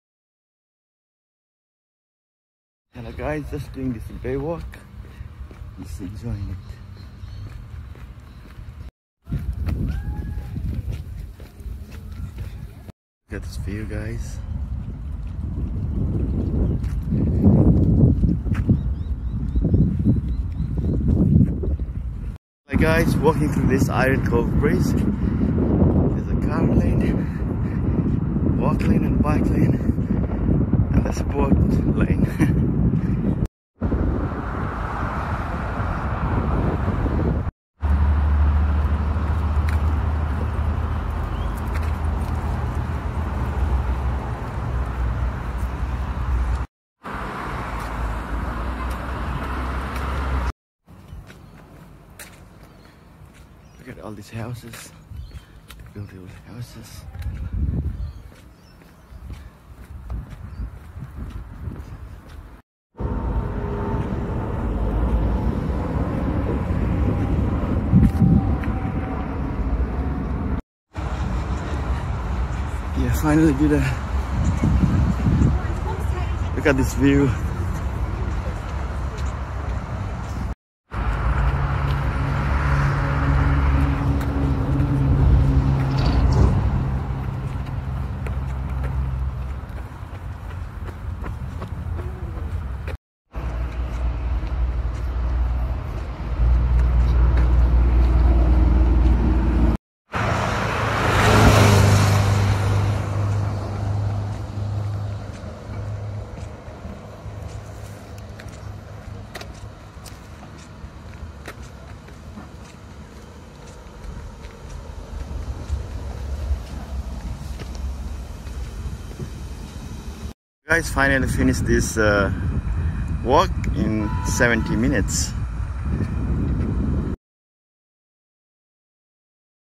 Bay Run ( walk) , what a beautiful view and day
Hello guys did this bay walk after probably 9 years and felt so good . Highly recommended I started from Leichhardt side , but you could start from any where and plenty of street parking around .